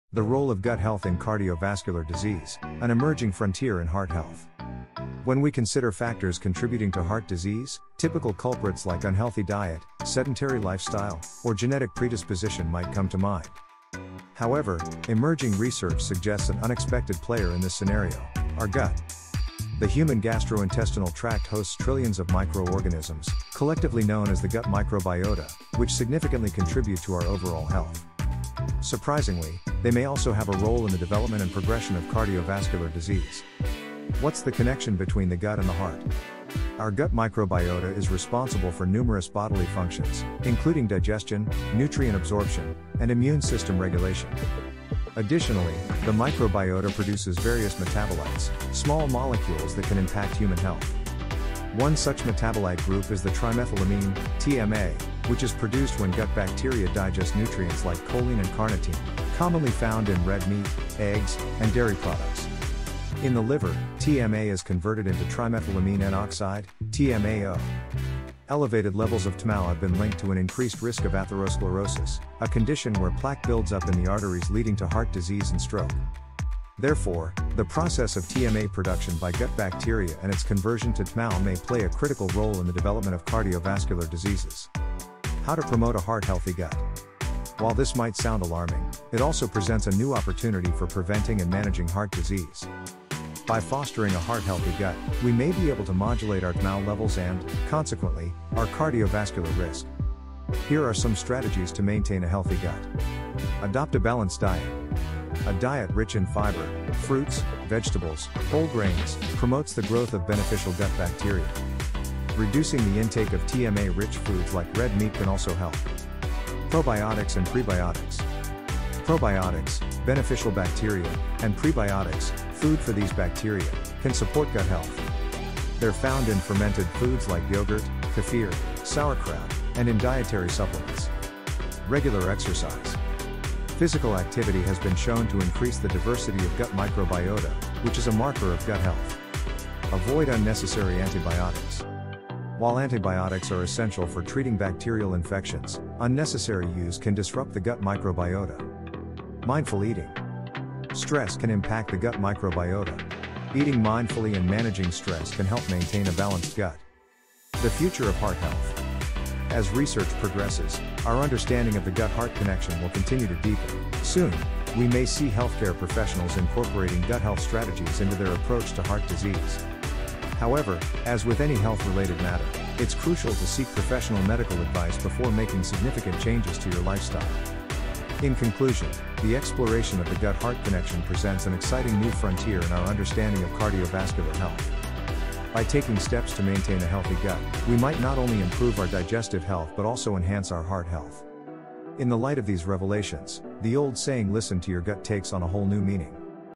The Role of Gut Health in Cardiovascular Disease An Emerging Frontier in Heart Health
When we consider factors contributing to heart disease, typical culprits like unhealthy diet, sedentary lifestyle, or genetic predisposition might come to mind.

However, emerging research suggests an unexpected player in this scenario - our gut.

The human gastrointestinal tract hosts trillions of microorganisms, collectively known as the gut microbiota, which significantly contribute to our overall health.

Surprisingly, they may also have a role in the development and progression of cardiovascular disease.